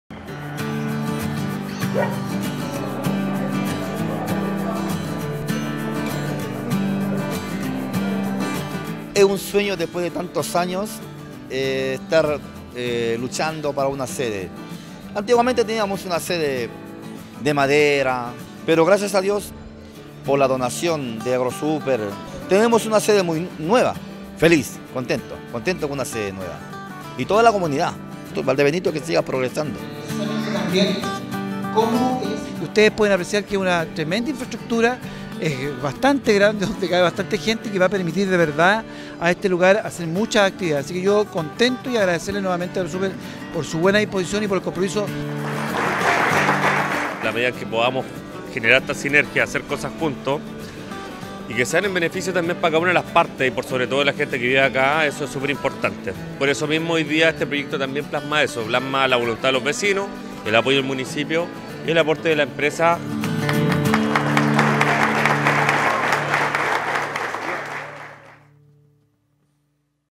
JUNTA DE VECINOS DE VALDEBENITO ALTO Y AGROSUPER INAUGURAN NUEVA SEDE SOCIAL
El nuevo lugar era un anhelo para esta comunidad, el que les permitirá mejorar las condiciones en las que realizan sus reuniones y actividades, beneficiando a los vecinos del sector y sus familias.